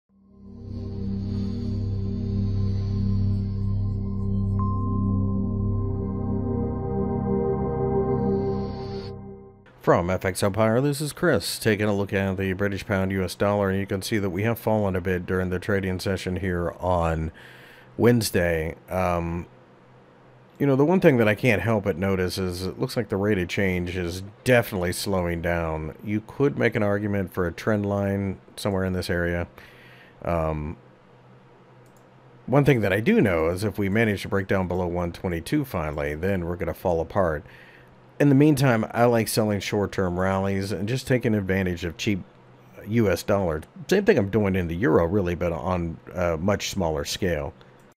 GBP/USD Technical Analysis for May 7, 2020 by FXEmpire
The British pound initially tried to rally during the trading session on Wednesday but then broke down a bit to show signs of exhaustion. Ultimately, we are still sitting above support though, so I do not necessarily believe that we are going to see some type of collapse in the short term.